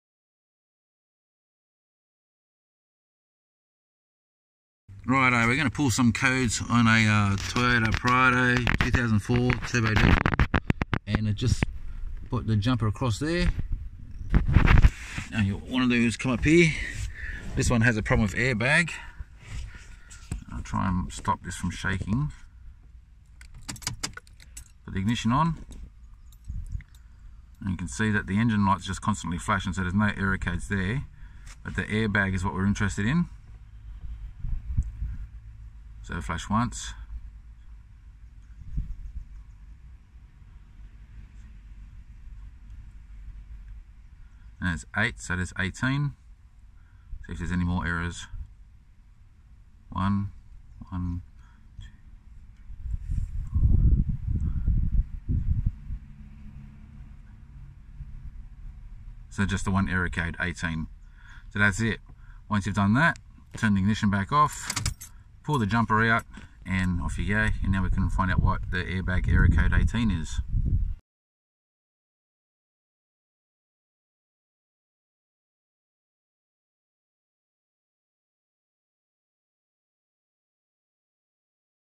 How to Pull Error Codes on a Toyota Prado 2004 | Found (Airbag Code 18)
No scan tool? No worries. Read your Prado’s fault codes in seconds. Simple jumper-wire trick anyone can do.

1. Make sure the ignition is OFF.
2. Turn the ignition ON (don’t start the engine).
3. Bridge pins **4 and 13** in the diagnostic connector with a jumper wire.
4. Read the codes displayed.
5. Turn the ignition OFF and remove the jumper wire.

Toyota fault codes are two-digit numbers.
• After shorting the pins, the first flash appears after about **4.5 seconds**.
• It will flash ON for 0.5 sec, OFF for 0.5 sec.
• Then the **first digit** of the code flashes.
• After a **1.5 sec pause**, the **second digit** flashes.

Example:
1 flash, 1.5 sec pause, then 8 flashes = **Code 18**.

If there’s only one code, there will be a 4.5 sec pause before it repeats.
If there are multiple codes, the pause between codes will be about **2.5 seconds**.

To clear codes from the ECU memory:
Remove the **ECD** fuse in the engine bay fuse box, leave it out for 60 seconds, and press the brake pedal once.
(If you disconnect the negative battery terminal instead, you’ll need to reset the clock, radio and window settings.)

**Update:**
The fault ended up being the clock spring. The owner ordered a new one and installed it himself — I was going to do the job, but he jumped in and sorted it.